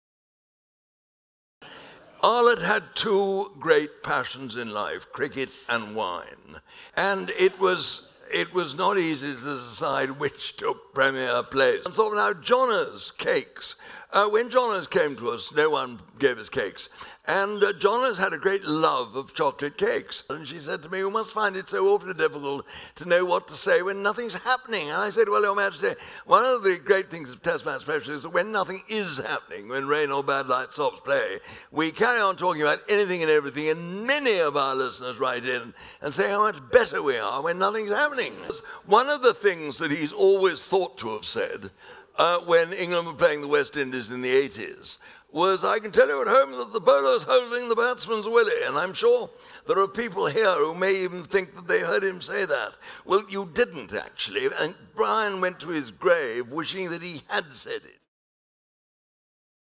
Simon Fielder Presents My Dear Old Things
Simon Fielder Presents Henry Blofeld in My Dear Old Things. Official promo for his 2023/24 tour.